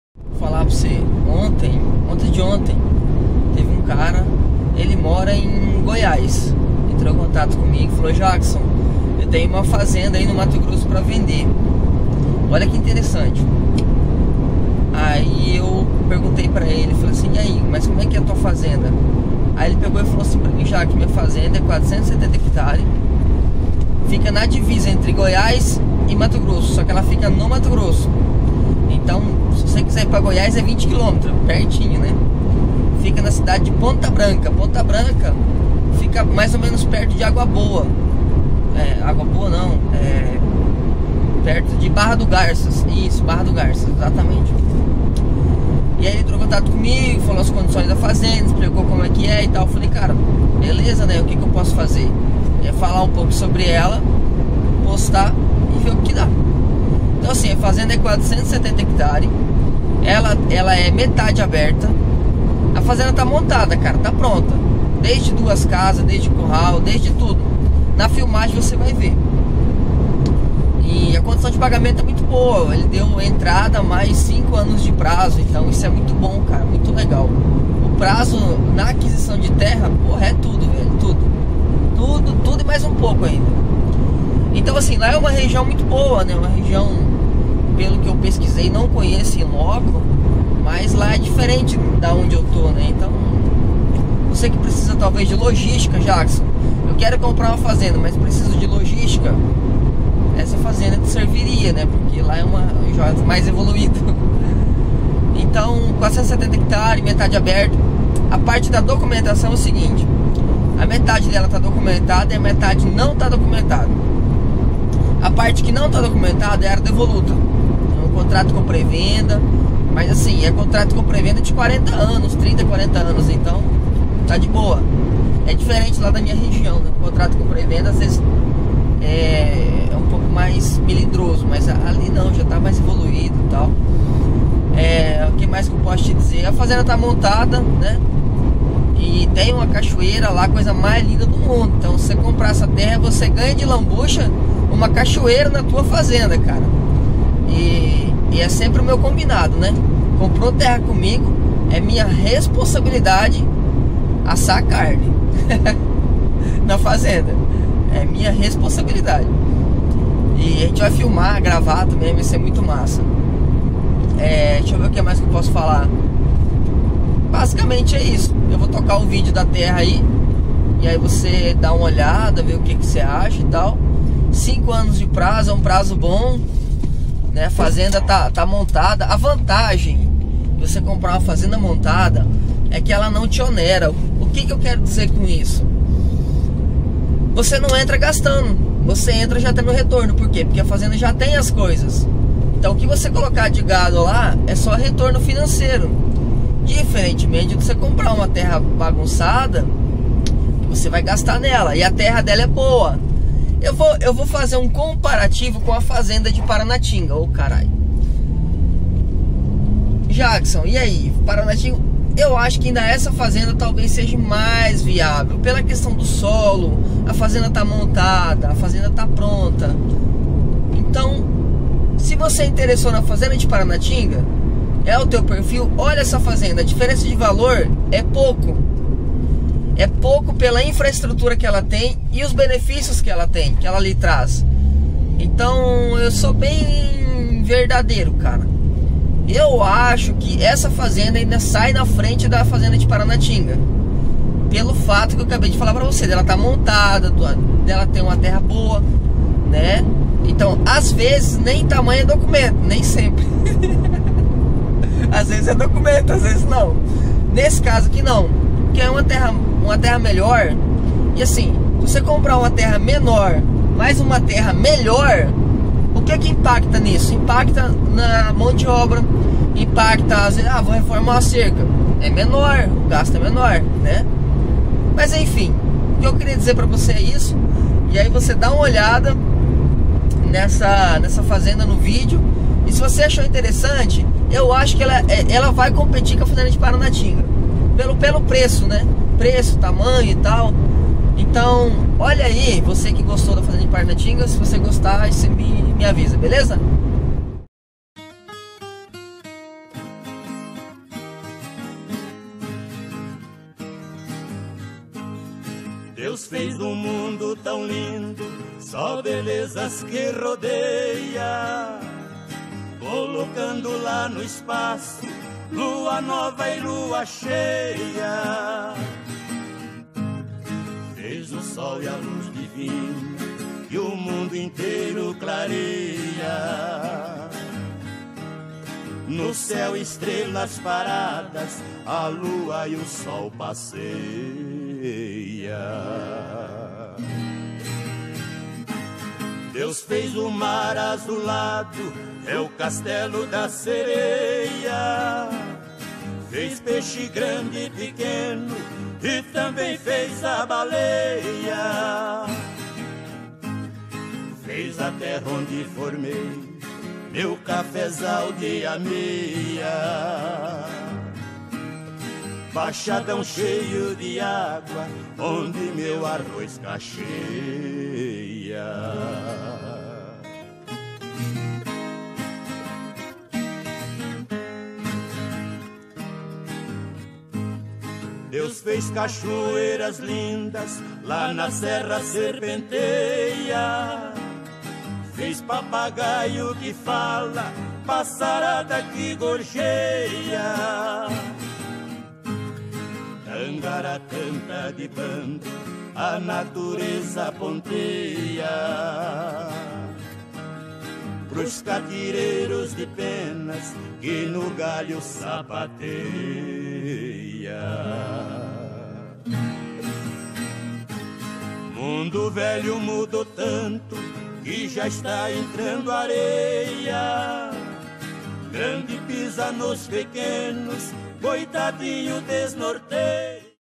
Fazenda em ponte branca MT 470 Hectares
Fazenda com metade aberta . A fazenda está toda montada e só chegar e mete gado